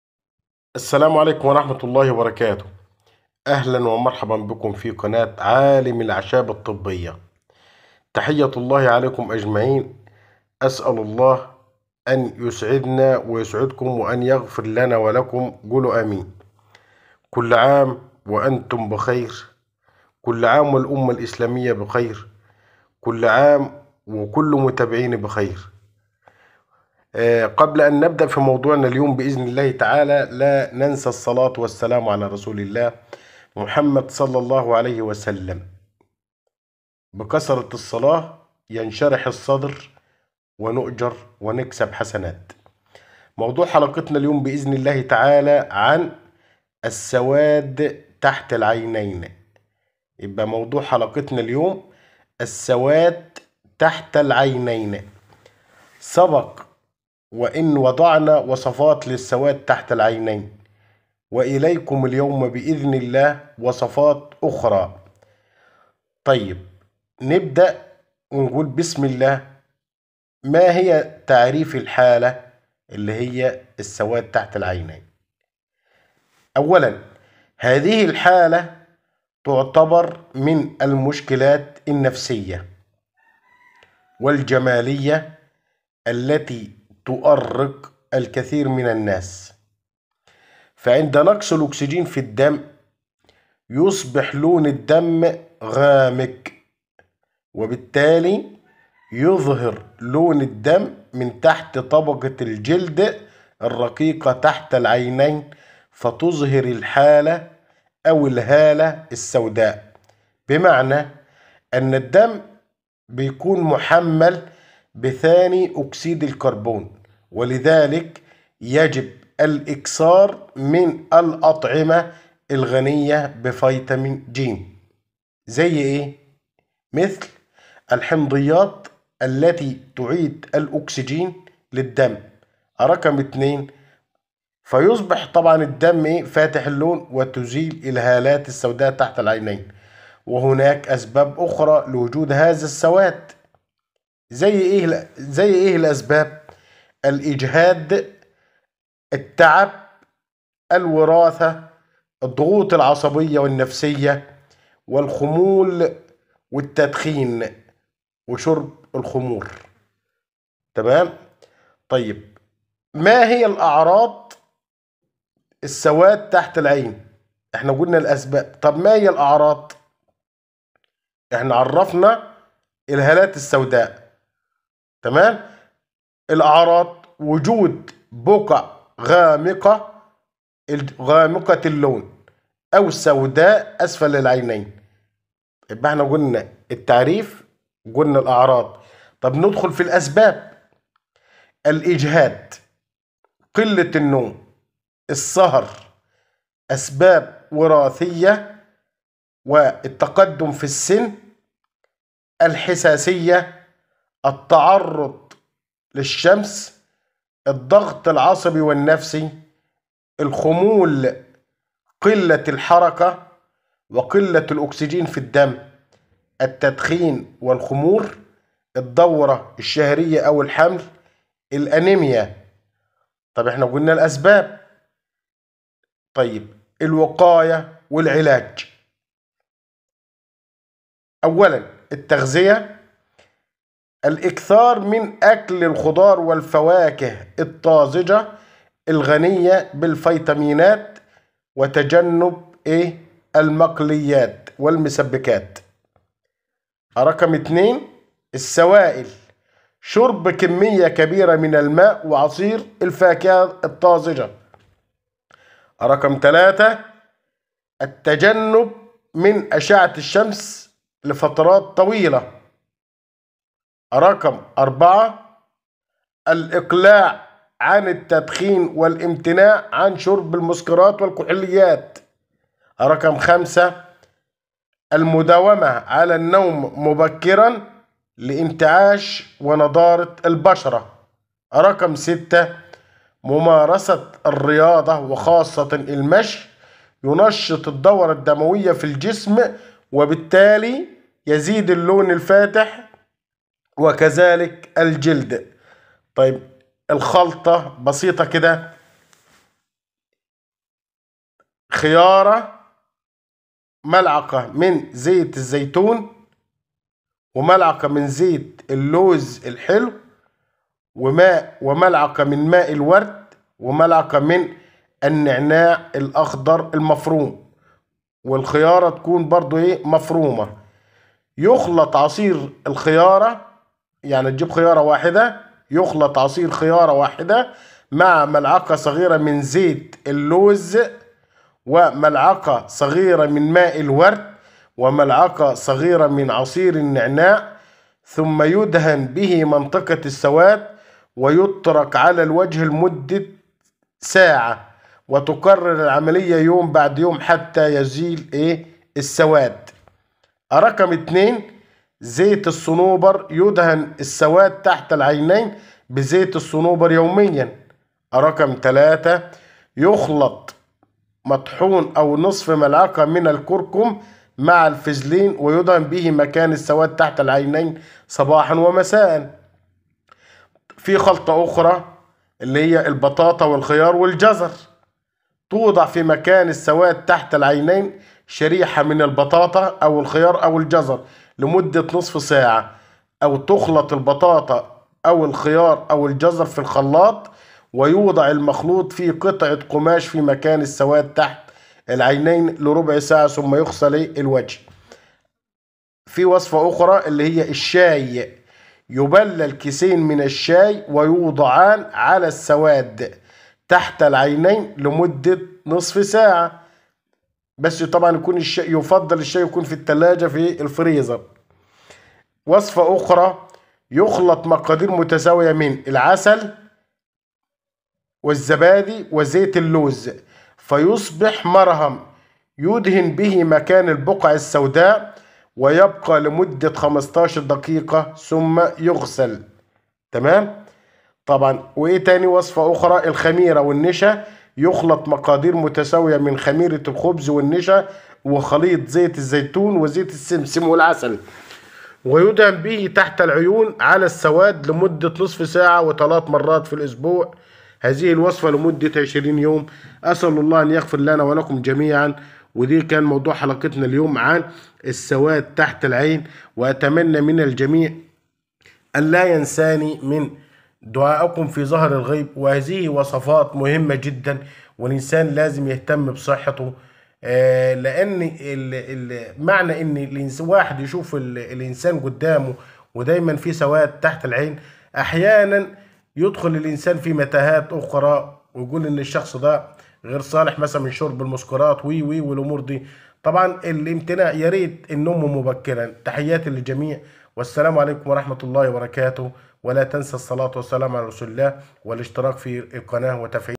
الهالات السوداء تحت العينين والعلاج المناسب
في هذا الفديو سوف نتعرف علي الهالات  السوداء
وطريقه العلاج المناسب والاعراض والتشخيص المناسب
السوادا تحت العينين .. ...👁

سبق وان وضعنا وصفات للسواد تحت العينين واليكم اليوم وصفات اخرى
لتعريف بالحالة :

هذه الحالة تعتبر من المشكلات النفسية والجمالية التي تؤرق الكثير من الناس ،
 فعند نقص الاوكسجين في الدم يصبح لون الدم غامق وبالتالي يظهر لون الدم من تحت طبقة الجلد الرقيقة تحت العينين فتظهر الهالة السوداء وهذا يعني ان الدم محمل بثاني اكسيد الكربون ولذا يجب الاكثار من الاطعمة الغنية
ب#فيتامين ج
مثل .
الحمضيات التي تعيد الاوكسجين للدم فيصبح فاتح اللون وتزول الهالات السوداء تحت العينين ، وهناك اسباب اخرى لوجود هذا السواد مثل الاجهاد والتعب والوراثة والضغوط العصبية والنفسية والخمور والتدخين وغيرها   .

وجود بقع غامقة اللون
او سوداء اسفل العينين   .

ا#الاسباب :  😳

الاجهاد وقلة النوم والسهر .
اسباب وراثية
والتقدم في السن     .
الحساسية    .
التعرض للشمس   .
الضغط العصبي والنفسي  .
الخمول وقلة الحركة  وقلة الاوكسجين في الدم   .
التدخين والخمور   .
الدورة الشهرية او الحمل   .
الانيميا   .

الاكثار من أكل الخضار والفاكهه الطازجة الغنية بالفيتامينات وتجنب المسبكات والمقليات   .

شرب كمية كبيرة من الماء وعصيرالفاكهه الطازج   .

 تجنب التعرض لاشعة الشمس لفترات طويلة   .

التدخين و
الخمور : 🍺🚬
الاقلاع عن التدخين والامتناع عن شرب المسكرات   .

 المداومة على النوم مبكرا لانعاش نضارة البشرة   .

الرياضة : 🏃‍♀️
ممارسة الراضة وخاصة المشي ينشط الدورة الدموية في الجسم وبالتالي يزيد الاوكسجين الذي يعطي الدم اللون الفاتح وكذلك الجلد   .

خلطه بسيطة. 🥣

الخيار .🥒
وزيت اللوز 🏺
وماء الورد ⚘
والنعناع☘ :

🔹️يخلط عصير خيارة🥒 واحدة مع ملعقة🥄 صغيرة من زيت🏺 اللوز  و ملعقة 🥄صغيرة من ماء الورد⚘ وملعقة صغيرة من عصير النعناع  ☘ثم يدهن به منطقة السواد ويترك على الوجة لمدة ربع ساعة وتكرر العملية يوم بعد يوم حتى زوال السواد  .

زيت الصنوبر : 🏺
يدهن السواد تحت العينين بزيت الصنوبر يوميا   .

الكركمين المطحون  :

🍠يخلط مطحون الكركم مع الفازلين🏮 ويدهن به مكان السواد تحت العينين صباحا ومساءا  .

الخلطة السحرية ...🥣

البطاطا 🥔
والخيار 🥒
والجزر🥕 :
توضع على مكان السواد تحت العينين شريحة من البطاطا🥔
 او الخيار 🥒
او الجزر🥕
 لمدة ربع ساعة ،
او تخلط البطاطا اوالخيار او الجزر في الخلاط ويوضع المخلوط في قطعة قماش على مكان السواد تحت العينين لربع ساعة ثم يغسل الوجه   .

الشاي :☕
 يبلل كيسين من الشاي ويوضعان على السواد تحت العينين لربع ساعة لشفاء السواد   .

العسل : 🍯
يخلط مقادير متساوية من العسل
 و🥛الزبادي
 و🏺زيت اللوز
 فيصبح مرهم يدهن به مكان البقع السوداء ويبقى لمدة 15 دقيقة ثم يغسل  .

يخلط مقادير متساوية  من خميرة الخبز 🍙والنشا 🍚
وخليط زيت _الزيتون🏺
 وزيت السمسم 🏮 والعسل 🍯
ويدهن منه تحت العيون على السواد لمدة نصف ساعة 3 مرات اسبوعا لمدة 20 يوم